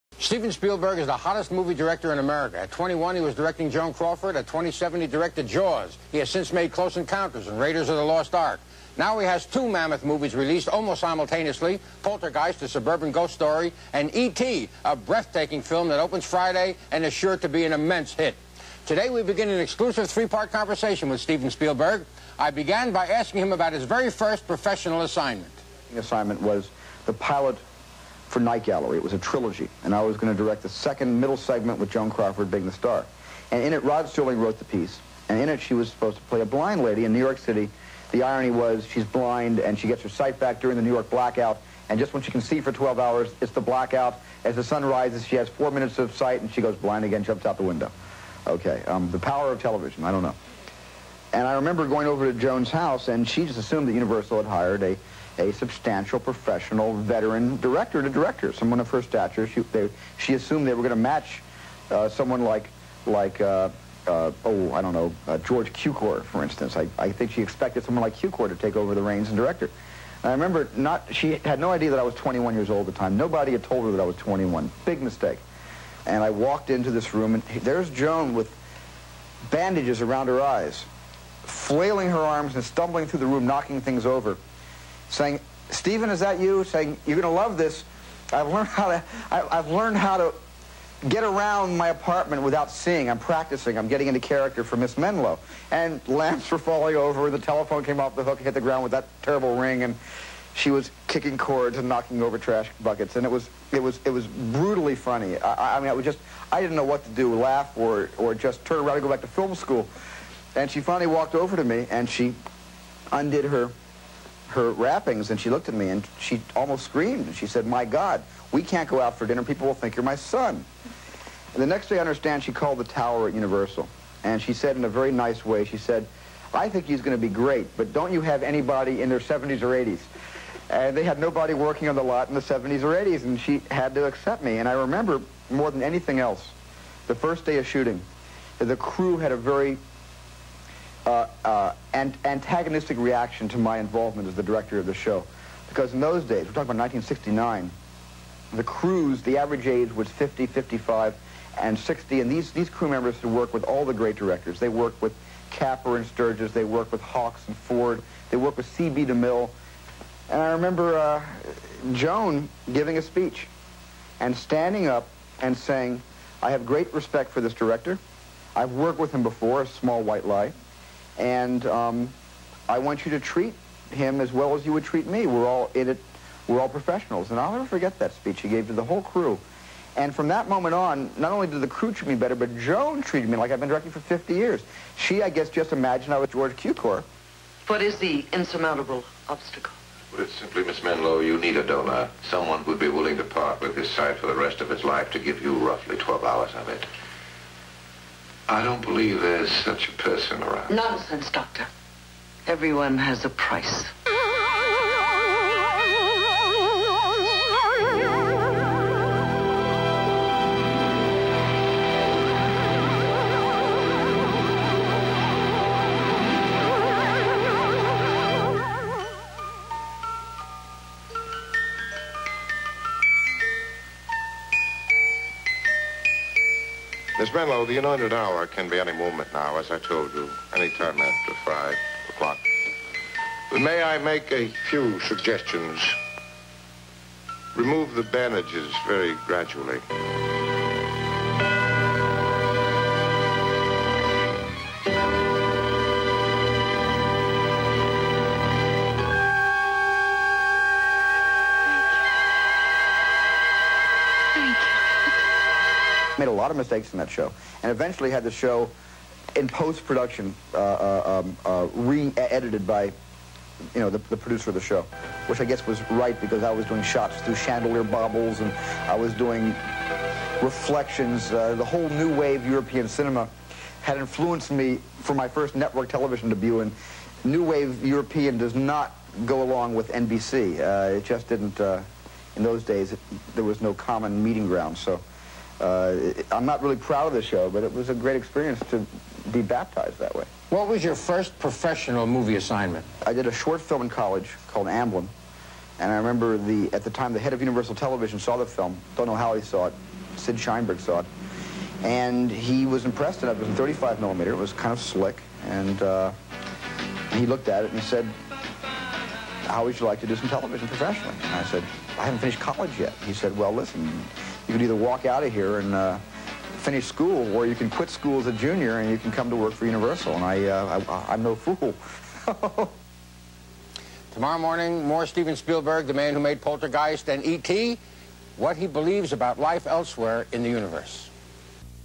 STEVEN SPIELBERG TALKS ABOUT HIS NIGHT GALLERY EXPERIENCE
Veteran Director Steven Spielberg talks about directing Joan Crawford in a Night Gallery episode.  1982